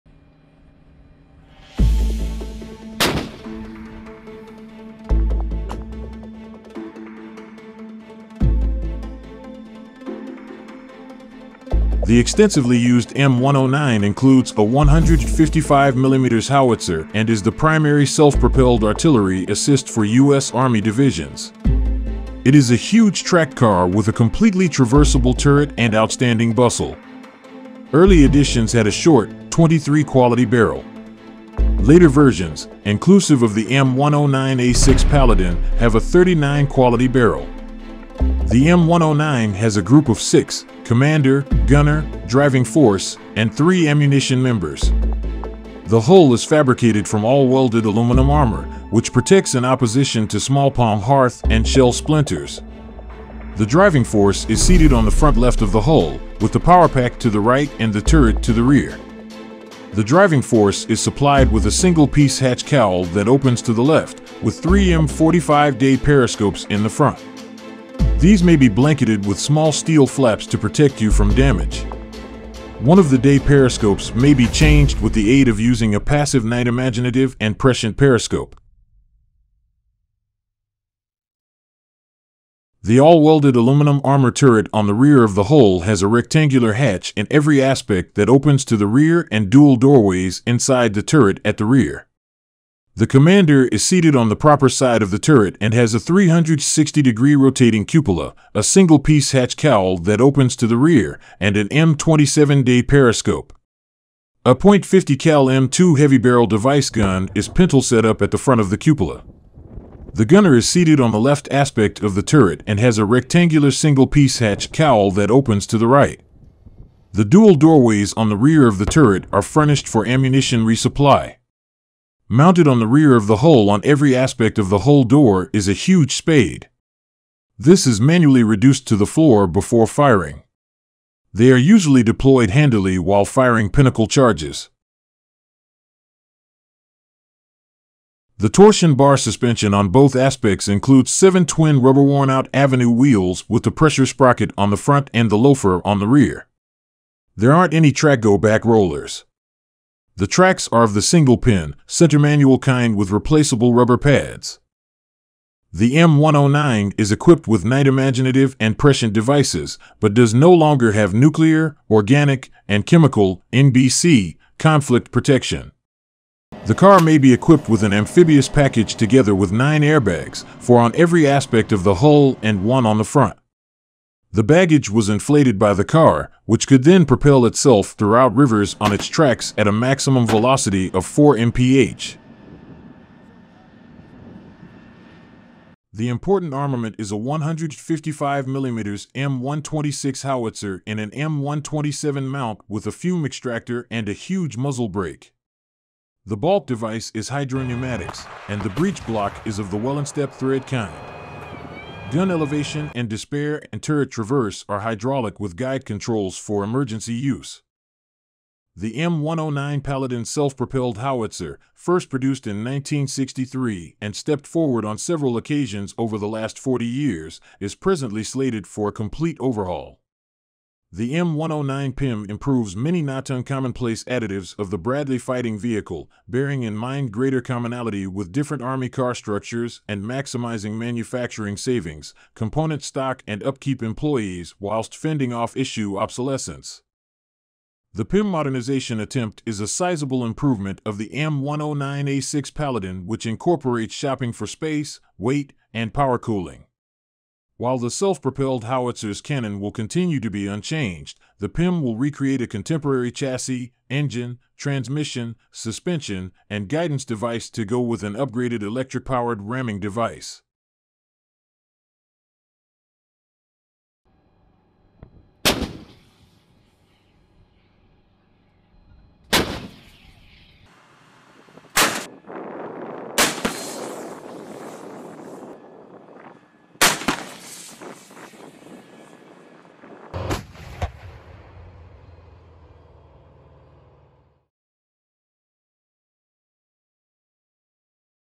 How does the M109 Paladin work?
The M109 is equipped with night-imaginative and prescient devices, but does no longer have nuclear, organic, and chemical (NBC) conflict protection. The car may be equipped with an amphibious package together with nine airbags, four on every aspect of the hull and one on the front. The baggage was inflated by the car, which could then propel itself throughout rivers on its tracks at a maximum velocity of four mph.